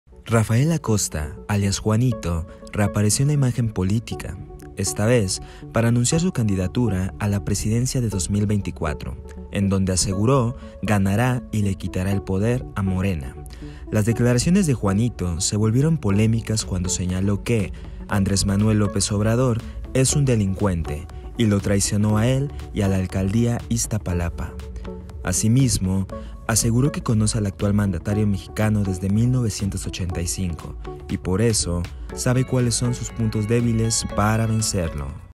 Juanito se apunta a la presidencia de 2024
Juanito volvió a la escena pública, está vez, para anunciarse como candidato presidencial y de pasó, criticar el gobierno de Morena.